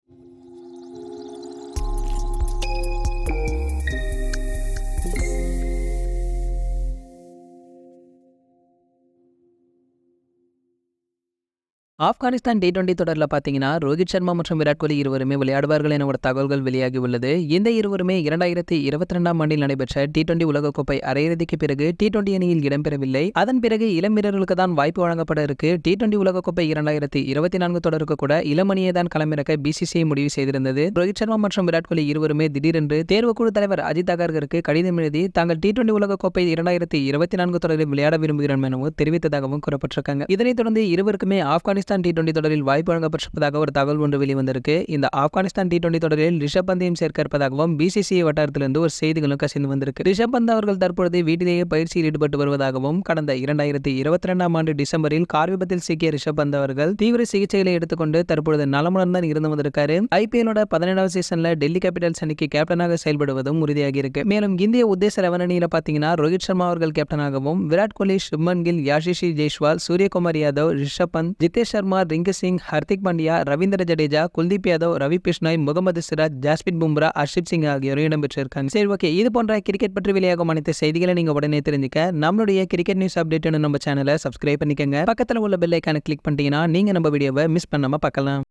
IND vs AFG: 'டி20 தொடர்'.. இந்திய XI-அணி இதுதான்? ரிஷப் பந்த் அதிரடி கம்பேக்? பிசிசிஐ அதிரடி முடிவு!
C R I C K E T __ N E W S __ U P D A T E S